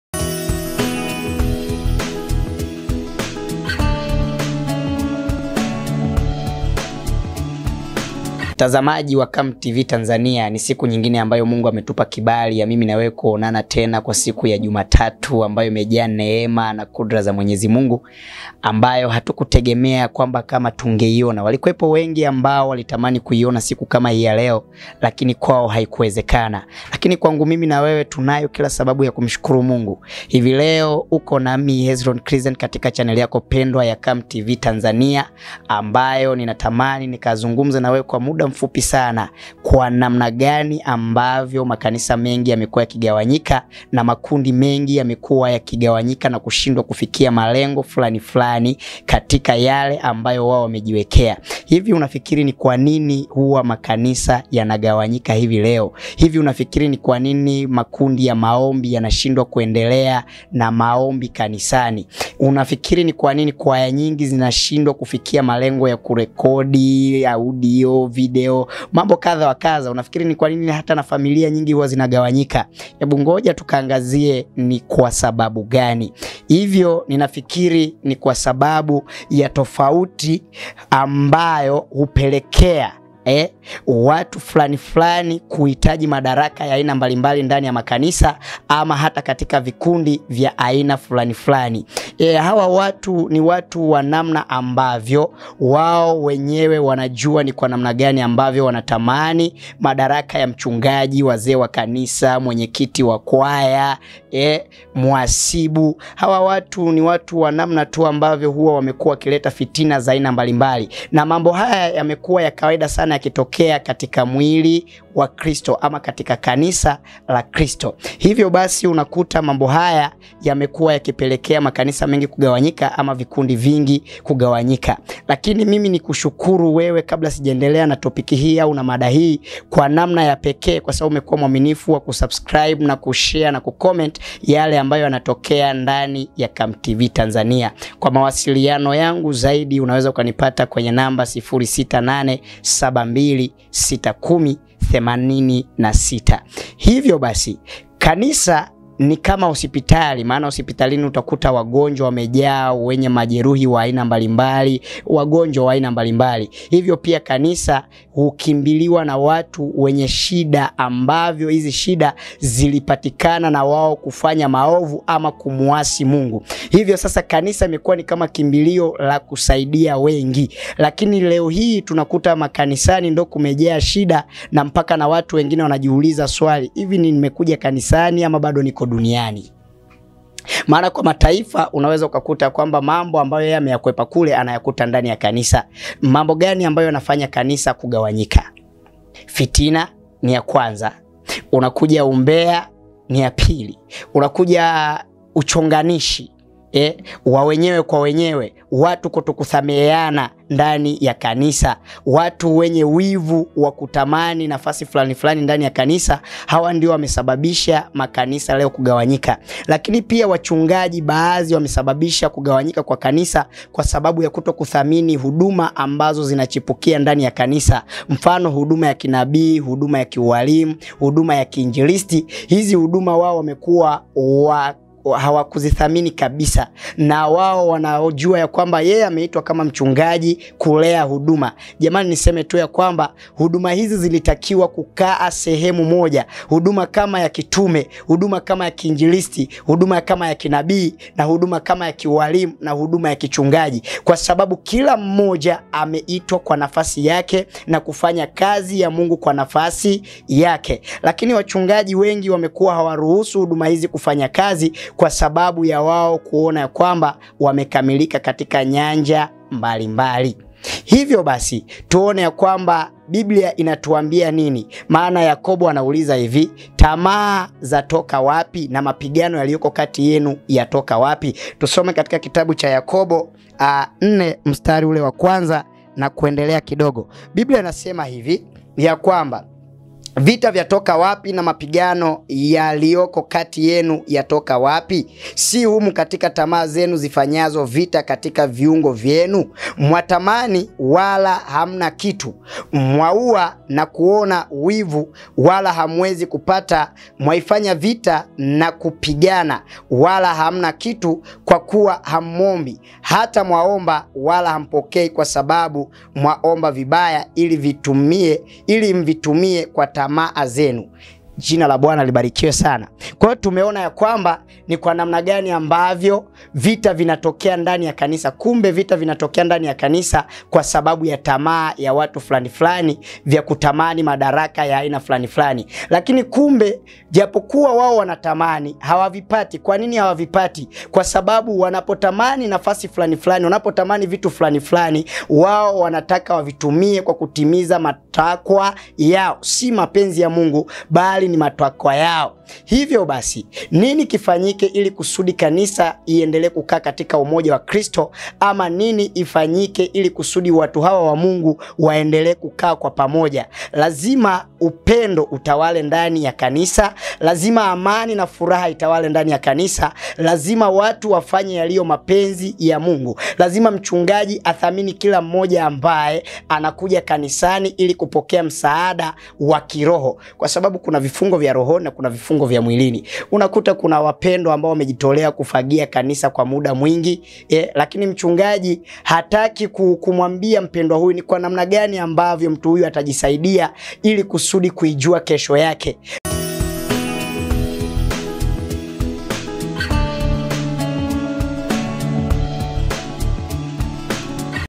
KUNASABABU NYINGI ZINAZOPELEKEA MAKANISA NA WATU KUGAWANYIKA.
...Tofauti ambazo hupelekea kanisa kugawanyika ni za huzuni na huwa ni za kawaida katika mwili wa Kristo. Matokeo ambayo huletwa na kugawanyika kwa kanisa bila kuzingatia ata sababu ya mgawanyiko huo huwa za uharibifu mkubwa kwenye kanisa. Mgawanyiko kwenye kanisa hueka waumini ambao wamekomaa kwenye dhiki na kufadhaika, na wale ambao ni wachanga hukata tamaa, pia husababisha maafa kwa wahubiri na familia zao na huleta lawama kwa jina la Kristo. Lakini kuna matumaini kwamba upendo unaeza rejea kwa kanisa ambayo imegawanyika.

Hosted_by Hezron Cresent
Supervisor by Mr.comtz